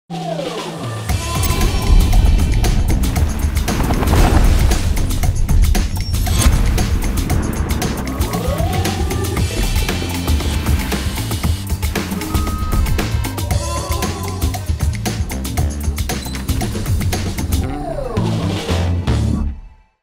Aspiradoras robot Gamma de Koblenz
Aspira cualquier superficie de manera práctica y rápida, la mejor limpieza con un clic y obtén más tiempo para ti.
Tecnología de mapeo inteligente con patrón de limpieza sistemático
Conexión WiFi, APP TUYA Smart con mapeo en vivo
Tecnología One Key Planning para memorizar rutas en tiempo real y navegar en áreas de manera inteligente cubriendo su hogar con rutas en forma de Z
Tres niveles de succión
Diseño esbelto para facilidad de acceso 6.8cm
Compatible con Alexa Amazon y Google Home
Potente motor de 1400pa, nivel de ruido bajo menor a 65dB
Control remoto multifuncional con pantalla LCD
Pared virtual, cinta magnética de 2.05m
Cepillo central con tecnología “V” libre de enredos
Función de programación de hasta 7 días
Auto recarga de batería, batería de litio de 2600 mAh